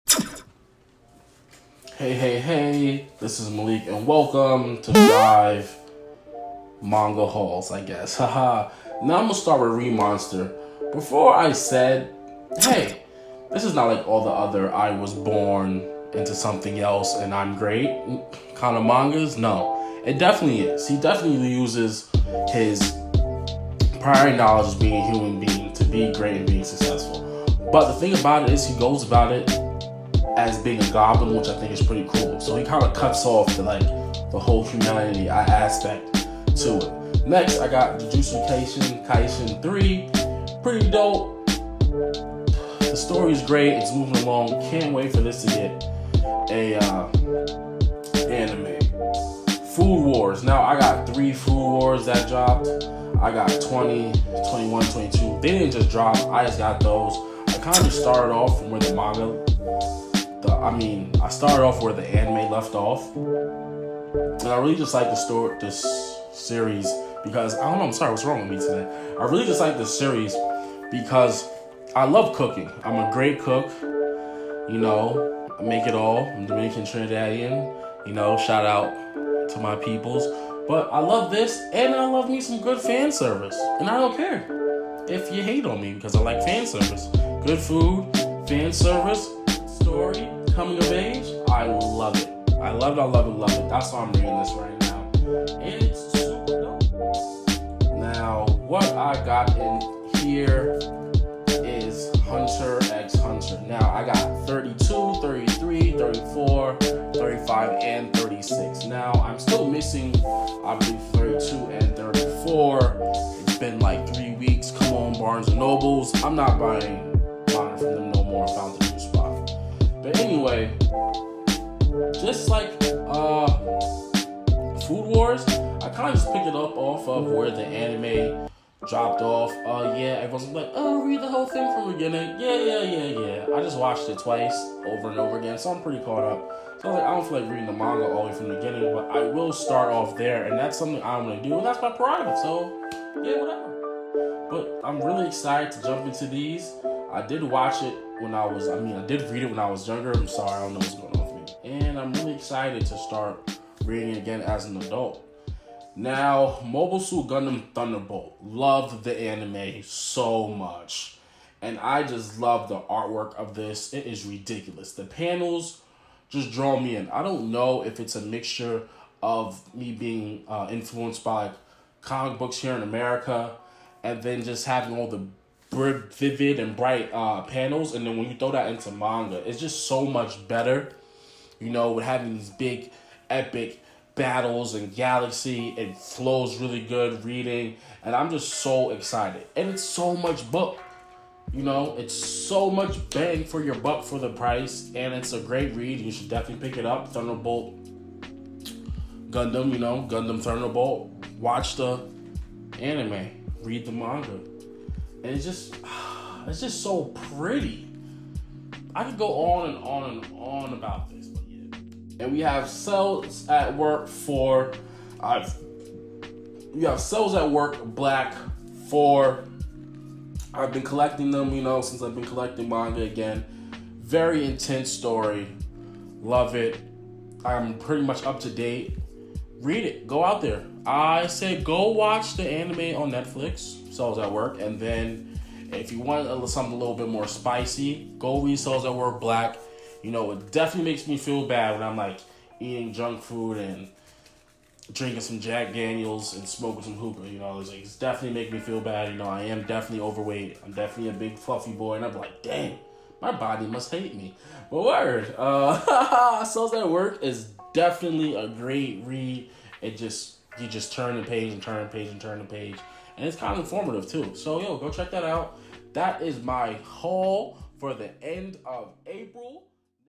FOOD WARS!, MOBILE SUIT GUNDAM THUNDERBOLT - April end of the month haul!
As the month comes to and end I did what? Buy more manga! I copped some Food Wars!, Jujutsu Kaisen, RE:MONSTER, Hunter x Hunter, Mobile Suit Gundam Thunderbolt. Check out what I think about these reads.